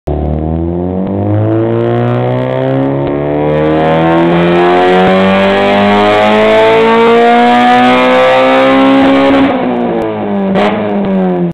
Honda Civic Sir @ Urban Per4mance [Bleeding Ears]
When the vtec kicked in my ears nearly exploded